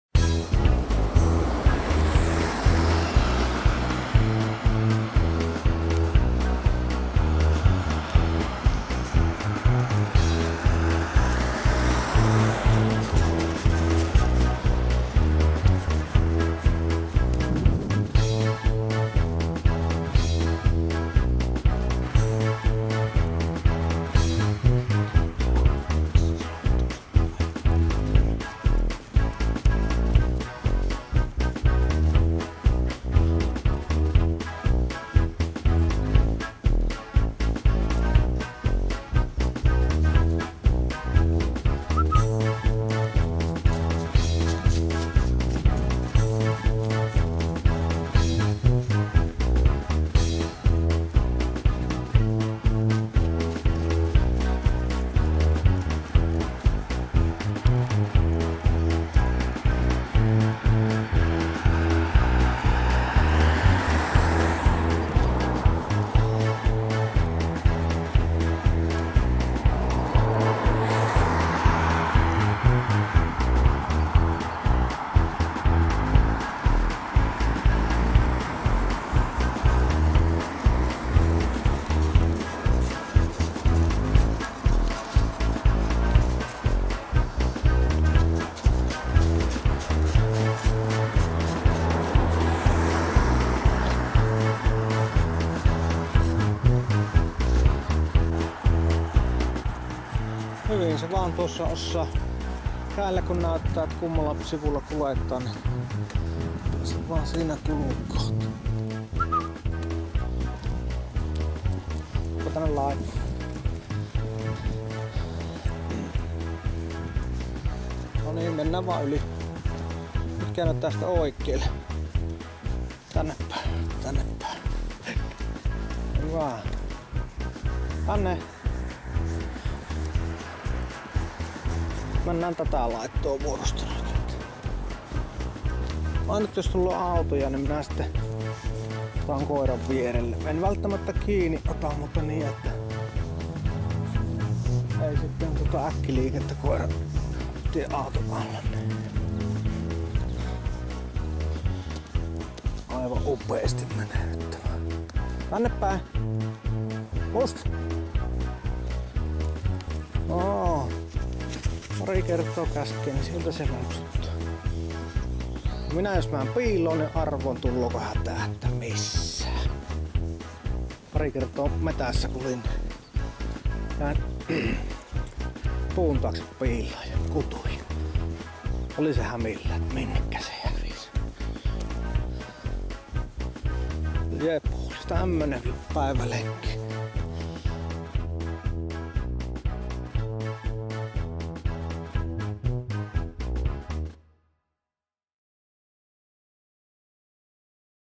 Musti vapaana lenkillä
Musti koiraani olen hiljalleen opettanut kulkemaan vapaana lenkillä ja näin meillä menee jo nyt.